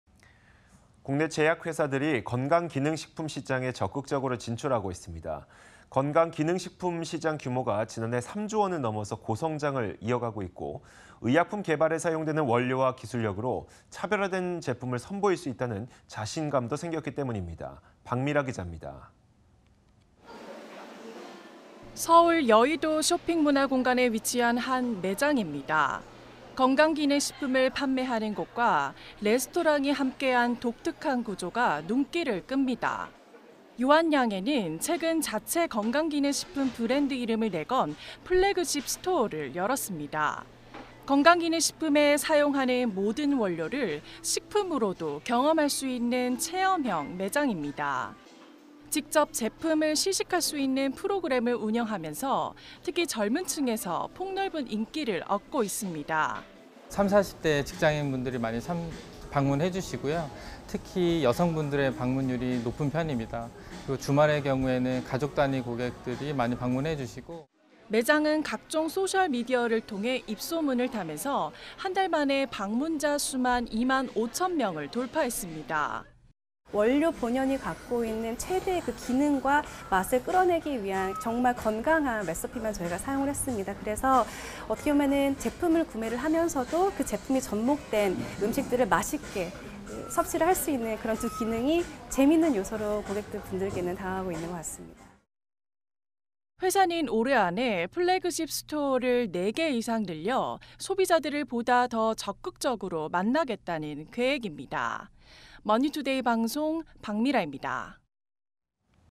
제약사들 '눈독'들이는 건강식품…"마케팅부터 남달라야"
06/05 MTN 투데이

국내 제약회사들이 건강기능식품 시장에 적극 진출하고 있습니다. 건강기능식품 시장 규모가 지난해 3조원을 넘어 고성장을 이어가고 있고, 의약품 개발에 사용되는 원료와 기술력으로 차별화된 제품을 선보일 수 있다는 자신감도 생겼기 때문입니다. 박미라 기자가 보도합니다.